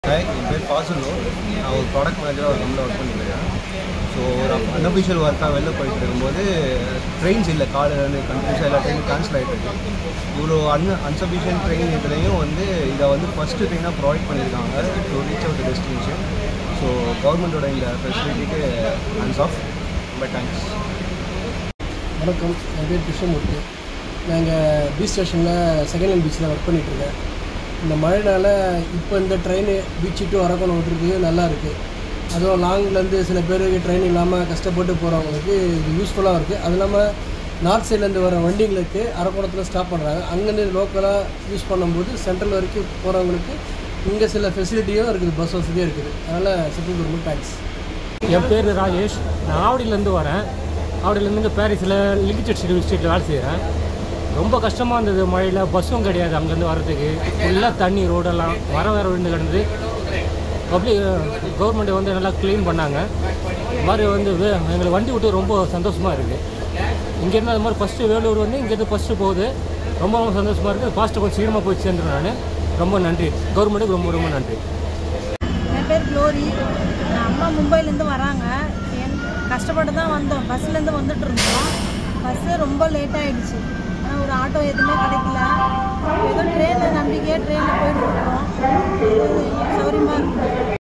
A sneak peek into passenger reviews on the restoration of EMU services in Chennai
A sneak peek into passenger reviews on the restoration of EMU services in Chennai. With Suburban trains back on track, commuters are enthusiastically sharing their experiences. 🚆🗣️